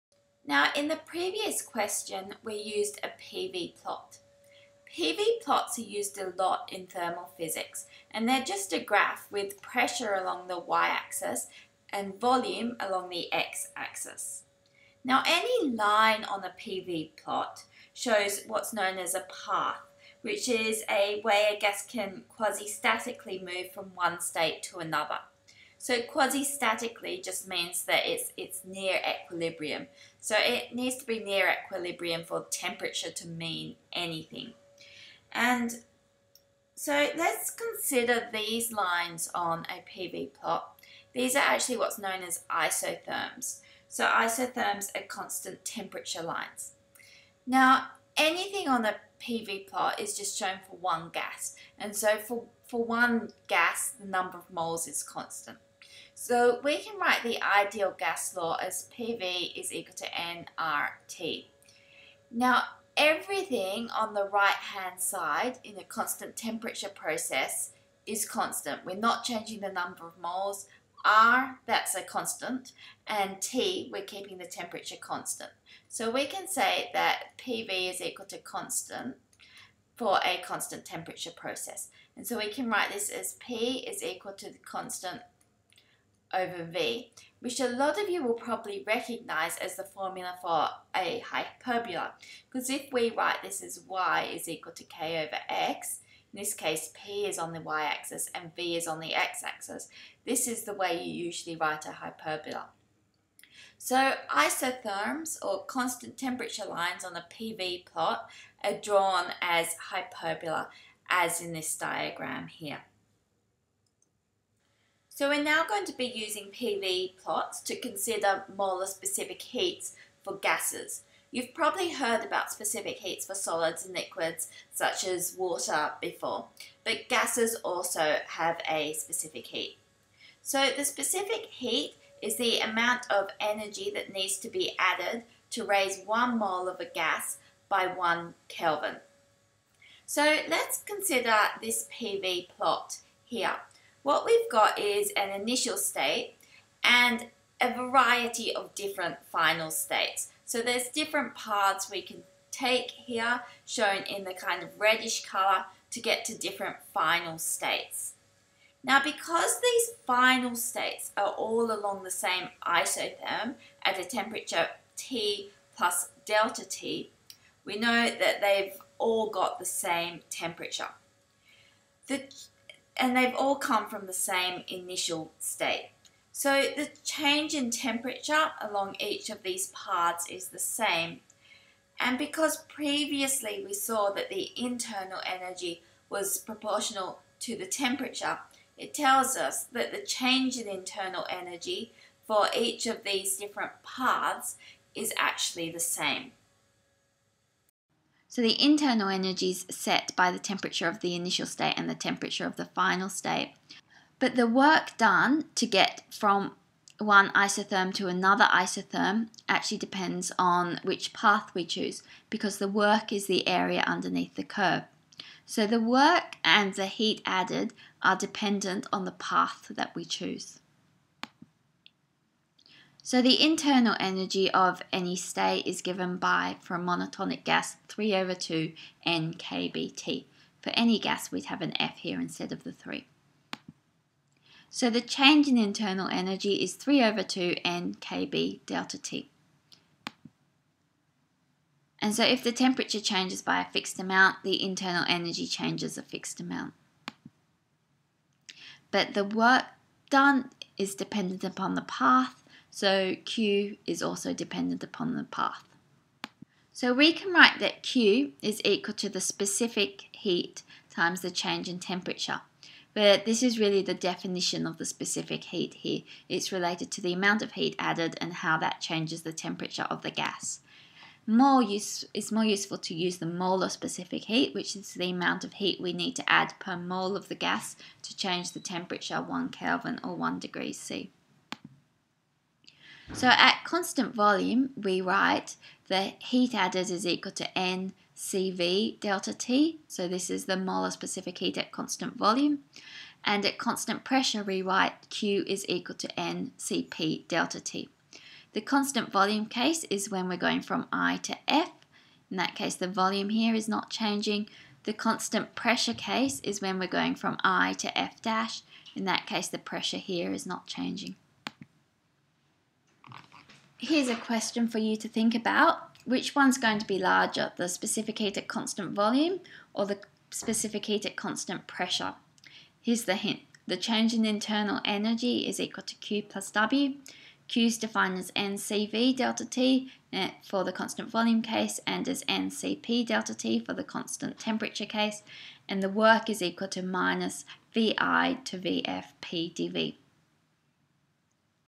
Introduction molar specific heat
This video will introduce you to molar specific heat.